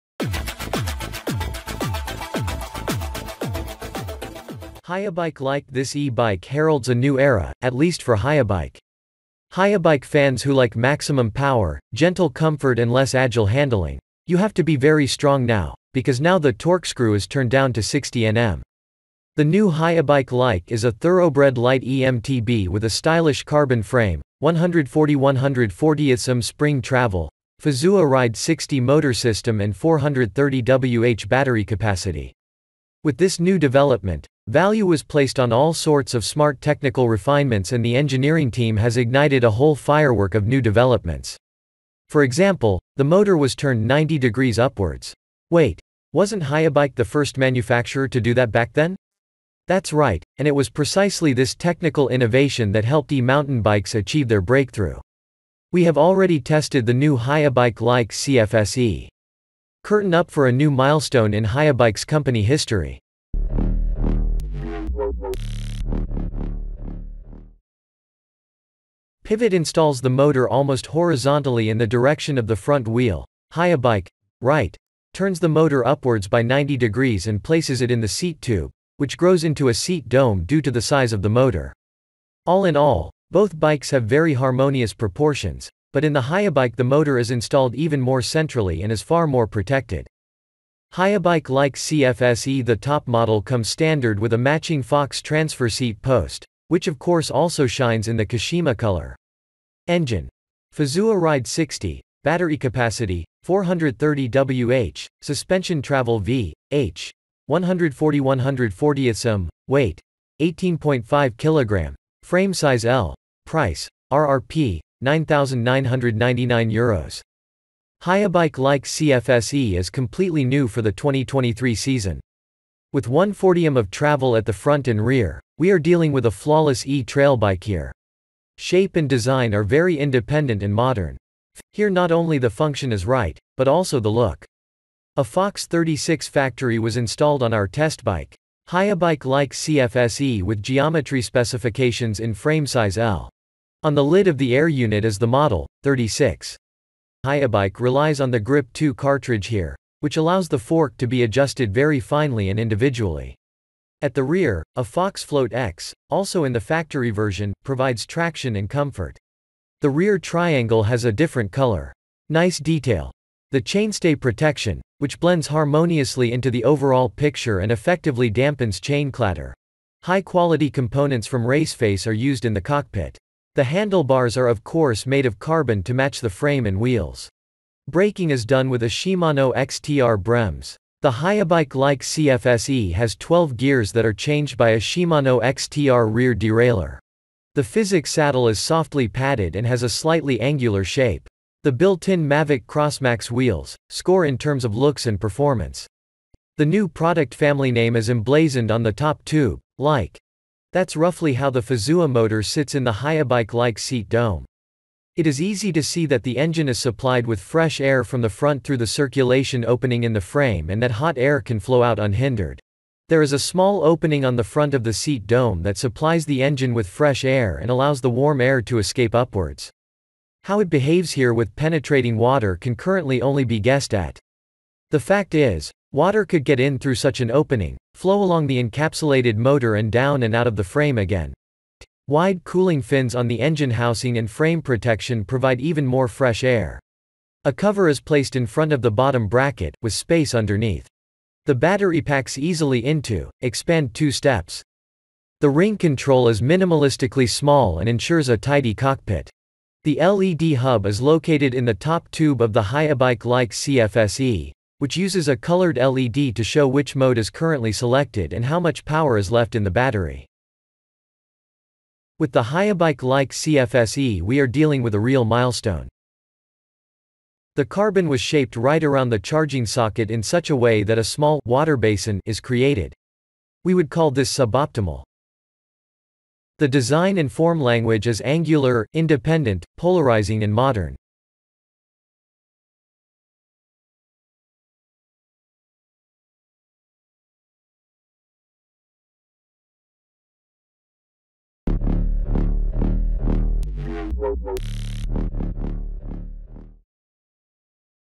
The New Haibike LYKE CF SE 2023 light eMTB with Fazua Ride 60 motor first ride
Light, lighter, Lyke: The Lyke CF SE. Our new Light eMTB line offers you a completely new riding experience and even more fun in terrain: With this carbon framed ultra-light, agile 29er all-mountain eMTB, you will experience an unprecedented trail ePerformance. The brand new, compact and extremely lightweight Fazua Ride 60 motor offers you just the right amount of support on the mountain. The innovative frame design of a closed downtube, and the unique placement of the motor in the seat tube, save excess weight and allow for a lower and centralised centre of gravity and efficient kinematics. The equipment of our Special Edition leaves nothing to be desired: Shimano XTR shifting and braking components, agile 140 mm Fox Factory suspension elements and no-compromise superior components make this high-end eMTB the object of desire for sporty eBikers who pay attention to every gram.